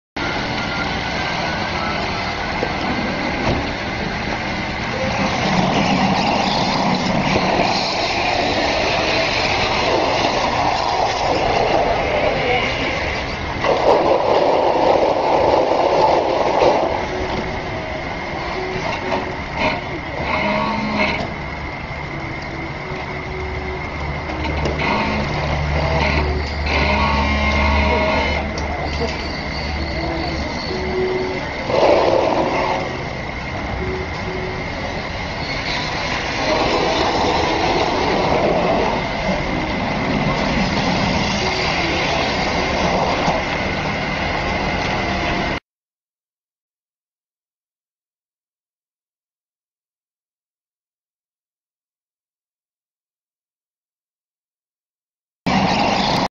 SUV ON TOP
Here is a clever driver who drive his geep on top and back